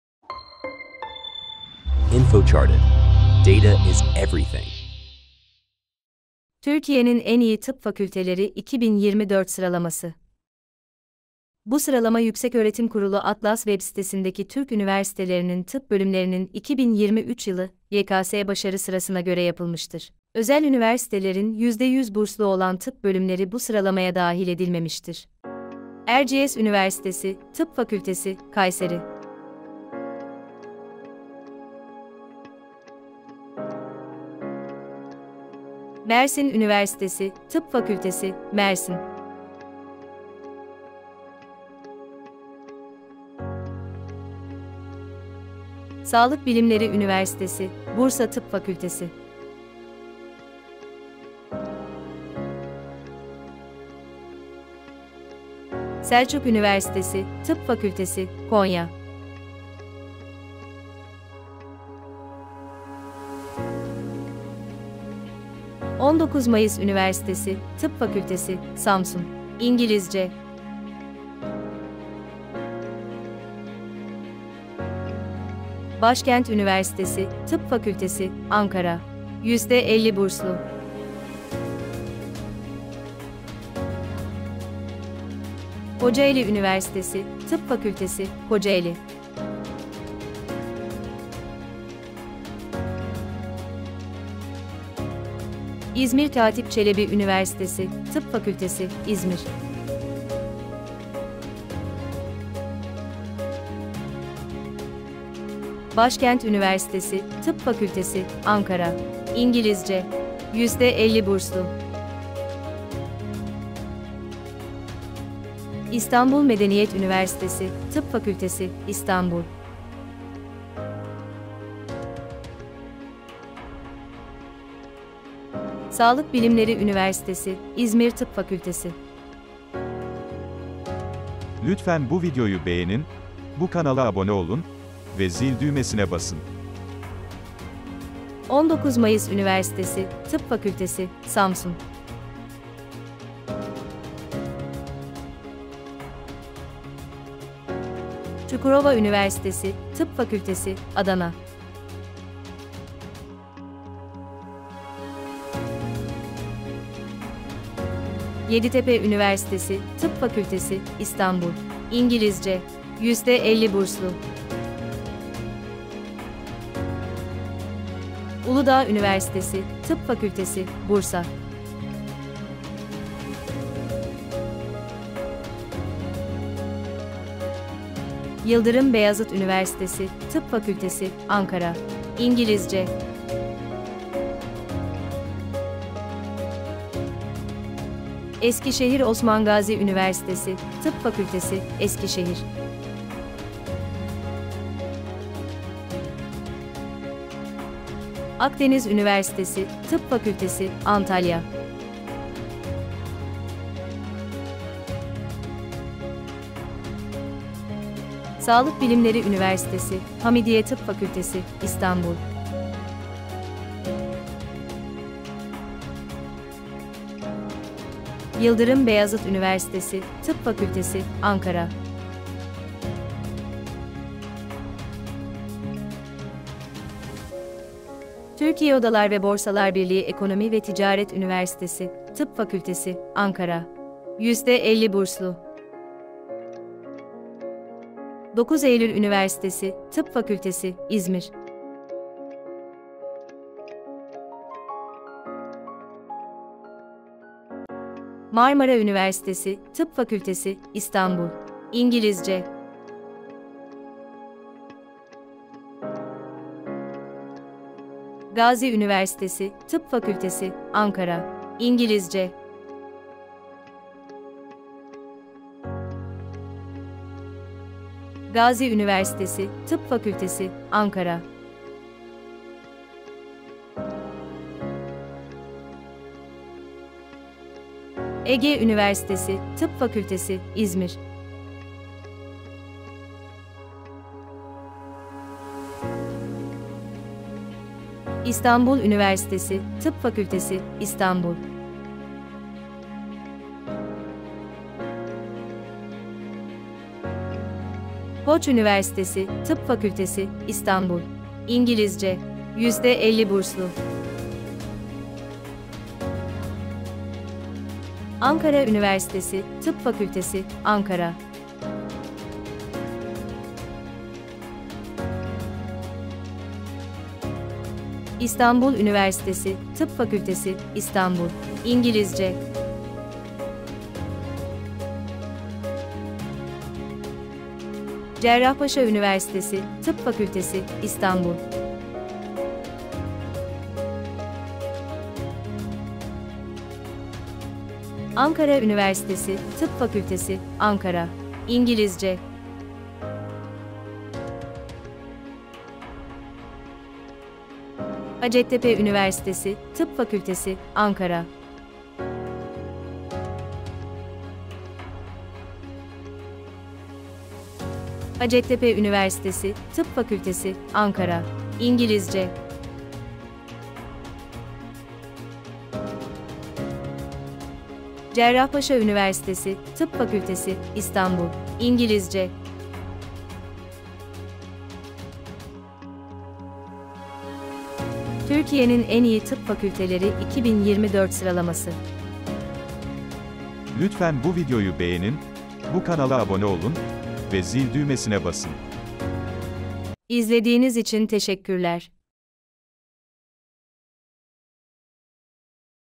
Türkiye'nin En İyi Tıp Fakülteleri 2024 Sıralaması
2024 yılındaki Türkiye en iyi tıp fakültelerini ve öne çıkan özelliklerini inceleyeceğiz. - Hangi tıp fakülteleri zirvede yer alıyor?- En iyi tıp fakültelerini seçerken nelere dikkat etmelisiniz? Geleceğinizi şekillendirecek en iyi tıp fakültesini bulun ve sağlık kariyerinize güçlü bir başlangıç yapın!